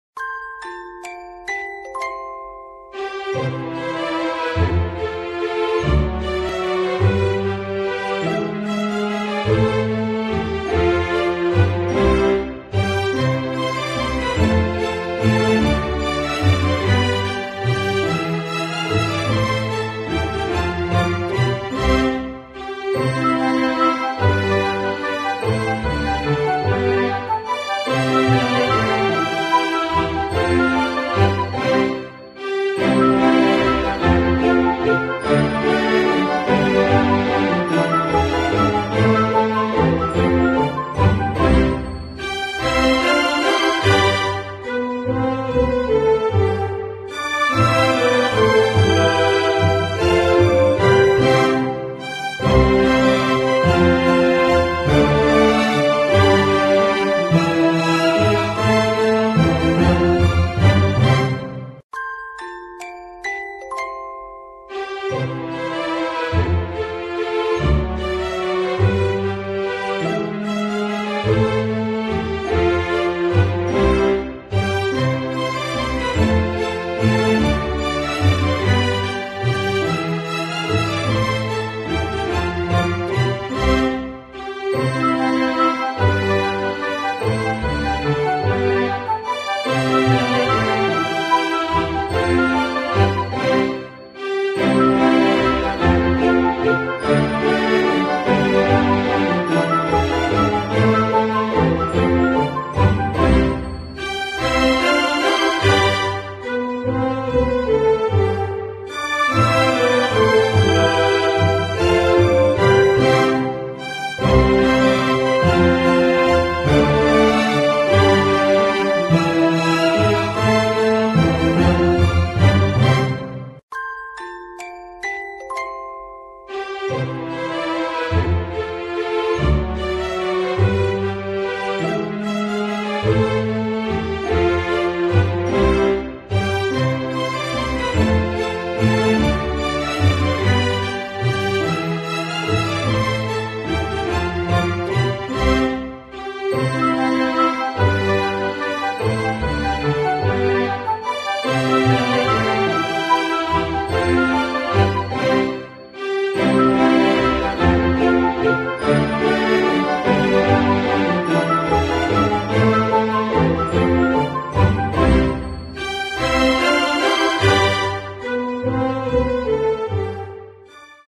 Dil Çubuğu ile Çam Ağacı Yapımı
Dil çubuklarıyla çok şık cam ağaçları yaptık.  Boyadık ve süsledik. Üçgen kavramını da pekiştirdik.